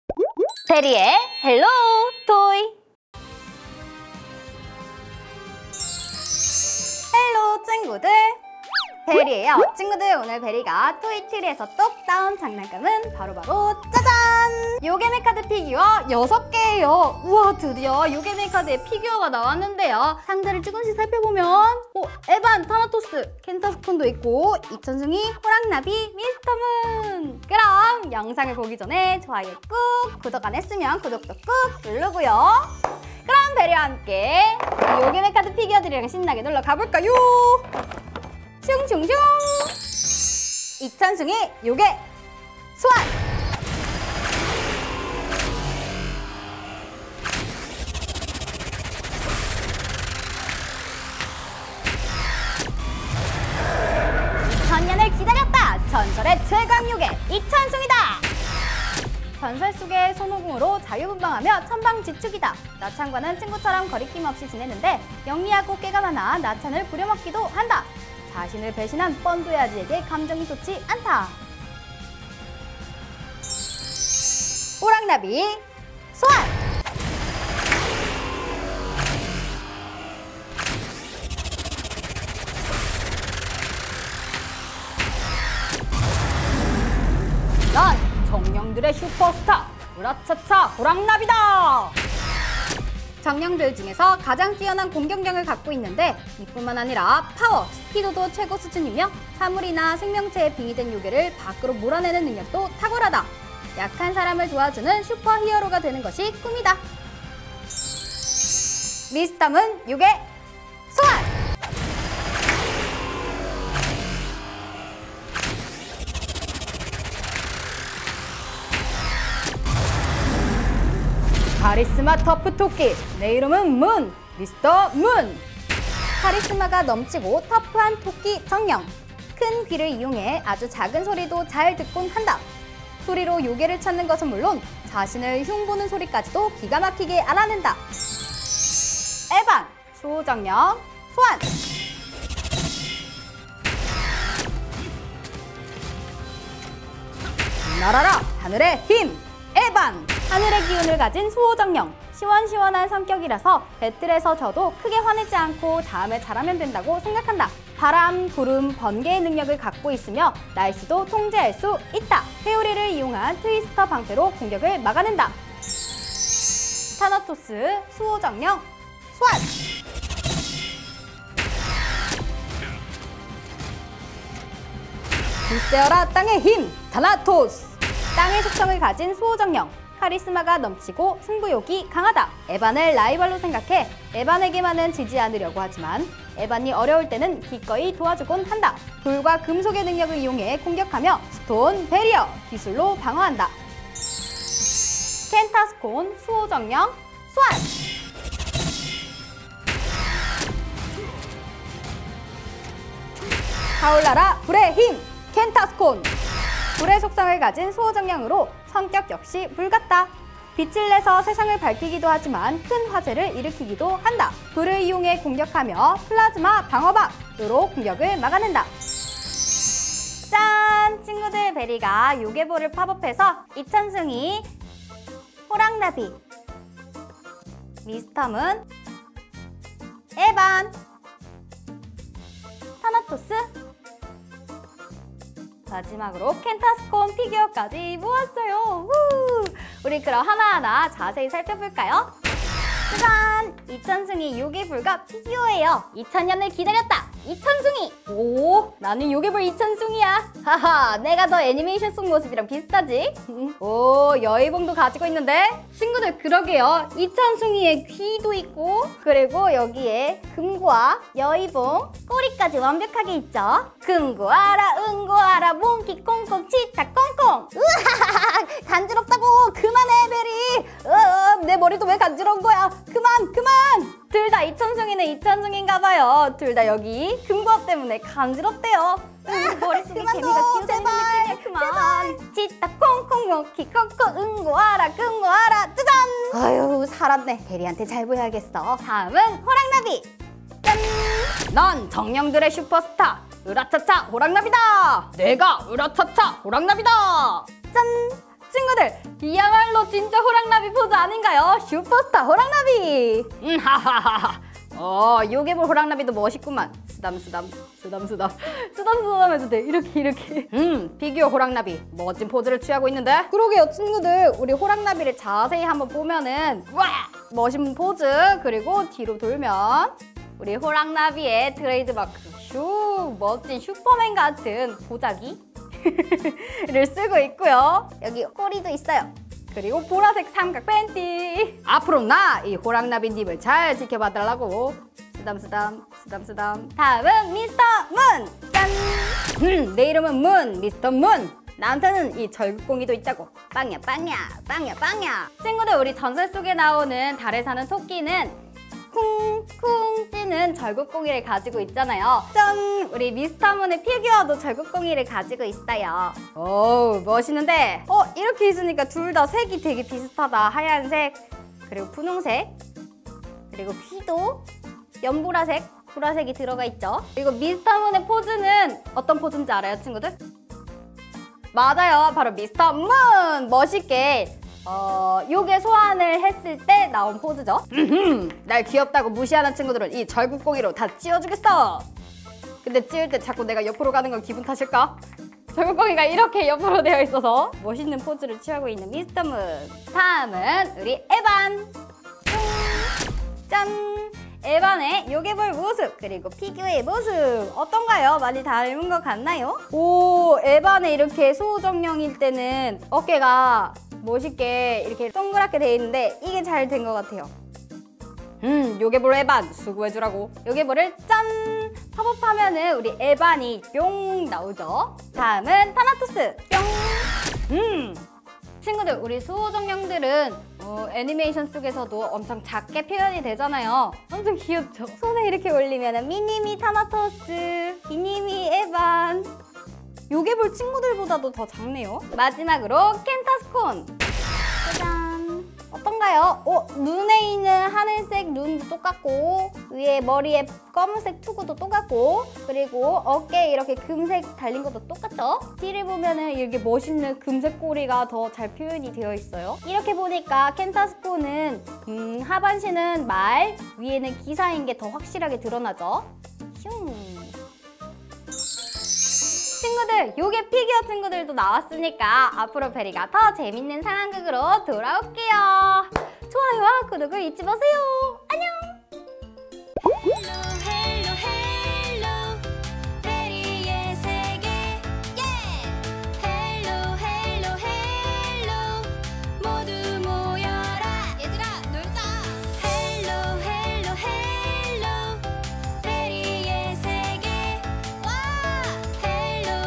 요괴메카드 피규어 이천숭이,호랑나비,미스터문,수호정령 에반,타나토스,켄타스콘 장난감 놀이 [베리]
헬로 친구들! 베리예요!
오늘 베리가 토이트리🌴에서 똑! 따온 장난감은 바로?!
요괴메카드 피규어 이천숭이,호랑나비,미스터문,켄타스콘,에반,타나토스입니다~!

전설의 최강 요괴 이천숭이! 정령들의 슈퍼스타 으랏차차 호랑나비! 카리스마 터프 토끼 내 이름은 문 미스터문!
날아라 하늘의 힘 엔반! 굳세어라 땅의 힘 타나토스! 불의 힘 켄타스콘!
그럼 베리와 함께 요괴메카드 피규어 6종을 만나러 가볼까요?! 슝슝슝=3=3